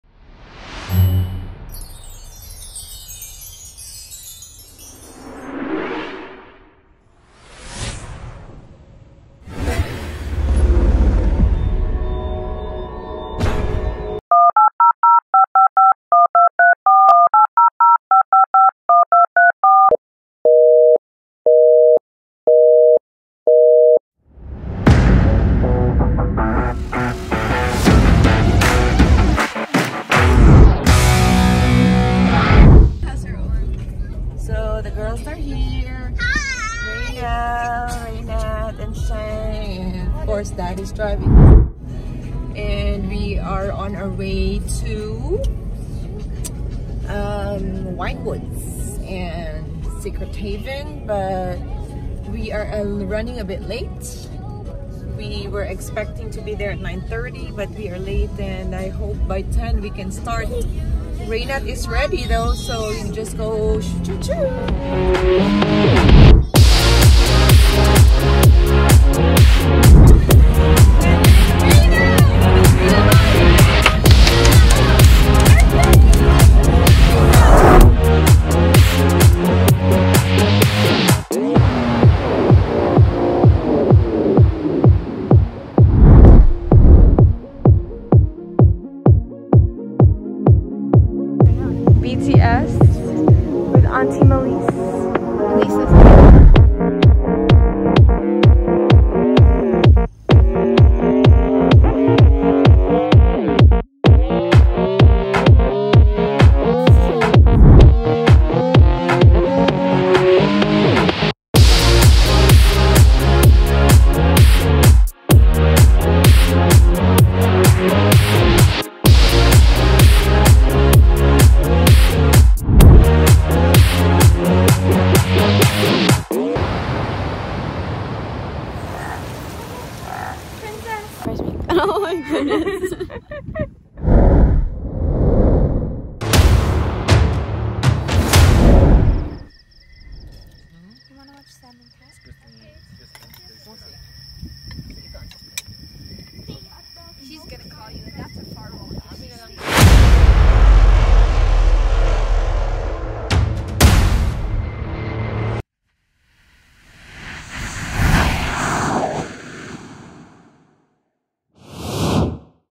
TWO YEARS BEFORE TWO DECADES
PRE DEBUT VIDEO & PHOTO SHOOT
BEHIND THE SCENES TEASER
A month from now, this young lady is turning 18, our 2nd daughter! The Lord will continue to be good and gracious to you. You will see and enjoy His favor all the days of your life... you are formed with a purpose and destined to reign! Keep chasing God and your dreams will chase after you...